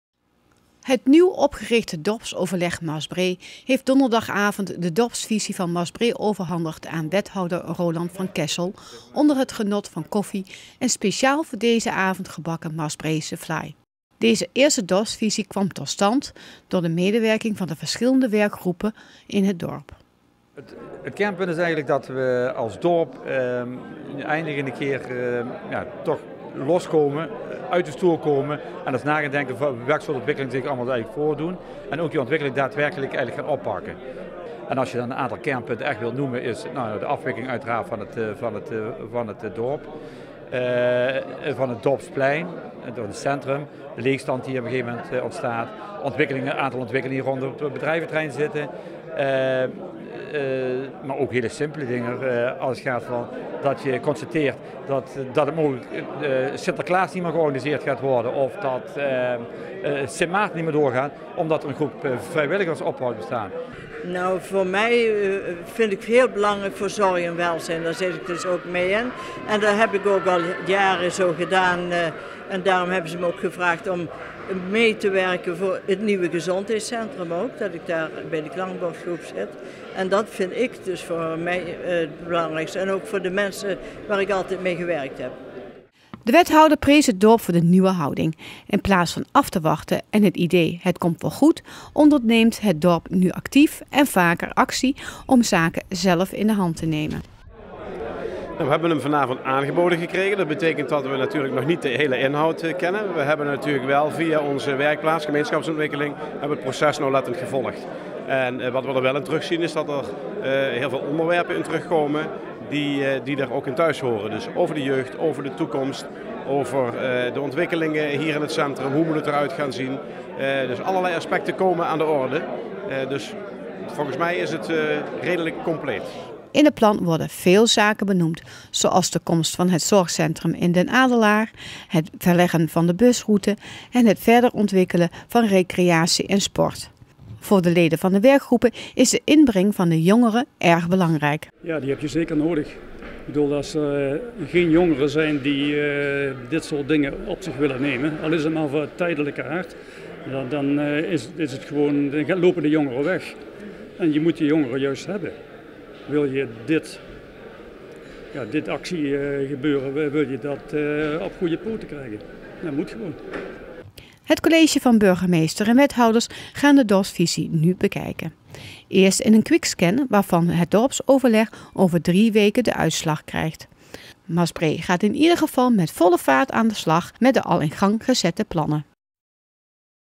Dorpsoverleg Maasbree biedt dorpsvisie aan
Het Dorpsoverleg Maasbree heeft donderdagavond de dorpsvisie van Maasbree overhandigd aan wethouder Roland van Kessel. De wethouder prees het dorp voor de nieuwe houding.

In plaats van af te wachten en het idee ‘het komt wel goed’, onderneemt het dorp nu actief en vaker zelf actie om zaken zelf in de hand te nemen.